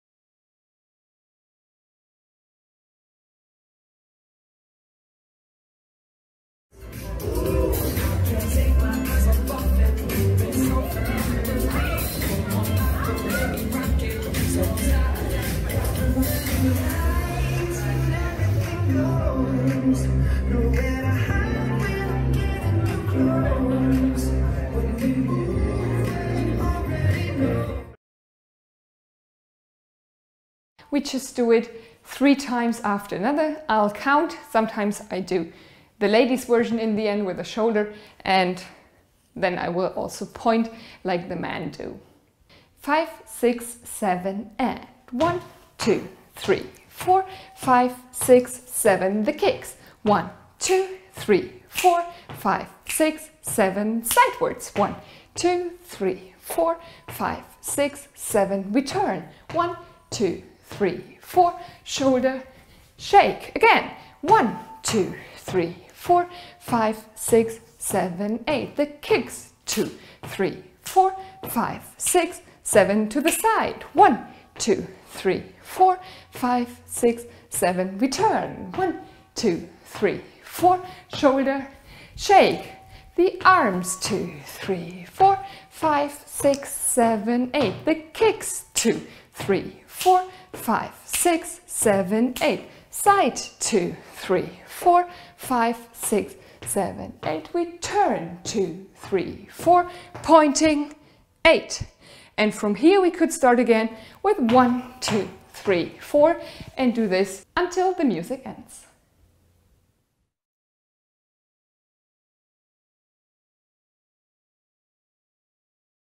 Flash mob 4: PREVIEW & QUICK LEARN TUTORIAL for flash mob/easy dance steps (4x8 counts)
Detailed step by step instructions, short video, long video to practice while watching, as well as videos with normal and fast music on our channel.